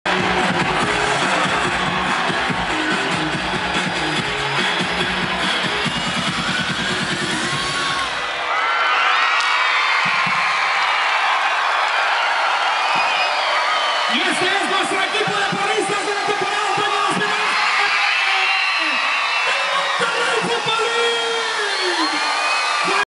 MFL Porristas
Todas las Porristas de la Monterrey Football League al final de su baile de la inauguración de la temporada MFL Otoño 2011.